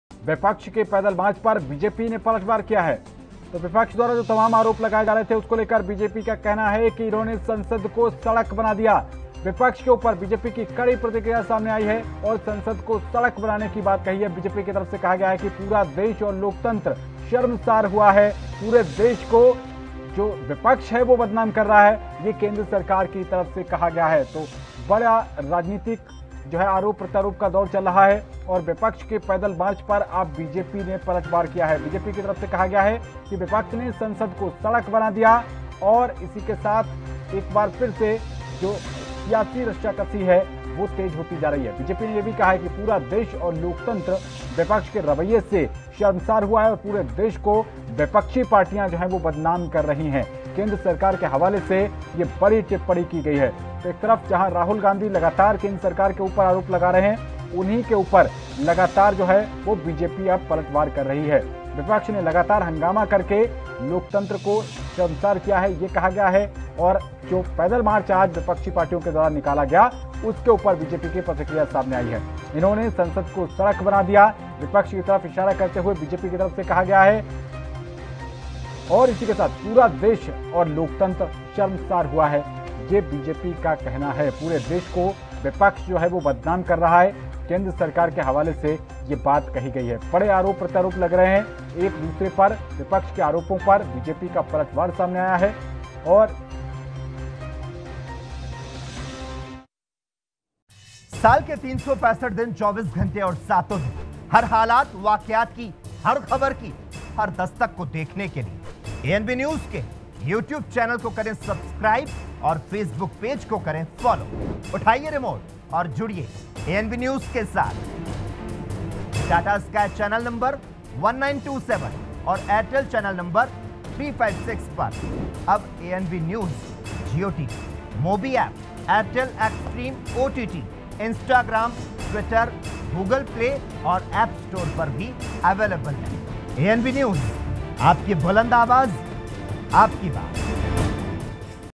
विपक्ष के पैदल मार्च पर BJP का पलटवार | ANB NEWS
AnbNews विपक्ष के पैदल मार्च पर BJP का पलटवार BJP ने कहा- इन्होंने संसद को सड़क बना दिया पूरा देश और लोकतंत्र शर्मसार हुआ- BJP